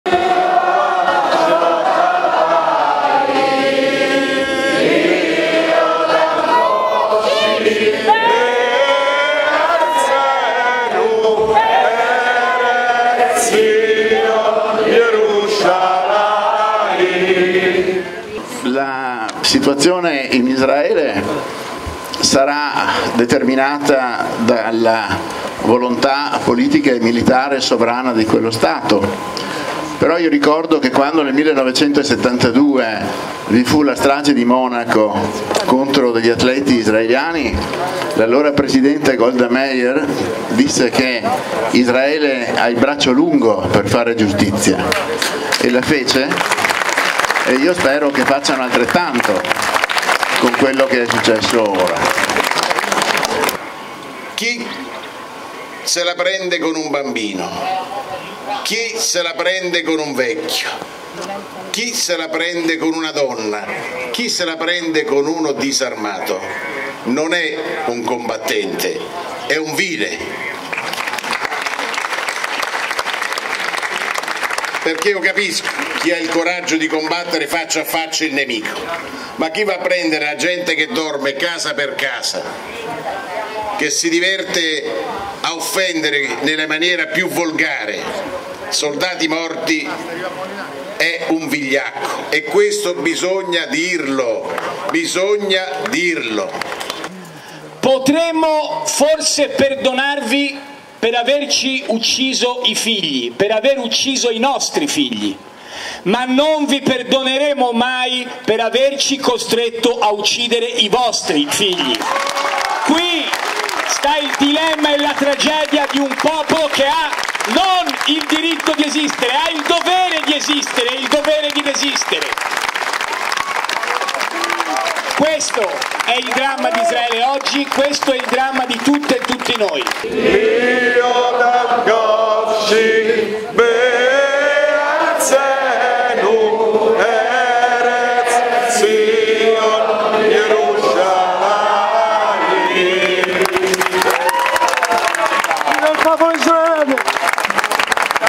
Israele, la fiaccolata all'arco di Tito, presenti i ministri Nordio e Tajani
Una fiaccolata per dimostrare vicinanza al popolo israeliano si è svolta nella serata di martedì 10 ottobre a Roma. Sotto l'arco di Tito si sono riuniti membri della comunità ebraica romana, ma anche esponenti delle forze politiche di maggioranza e opposizioni, oltre a membri del Governo quali il Ministro degli Esteri Antonio Tajani e il Ministro della Giustizia Carlo Nordio.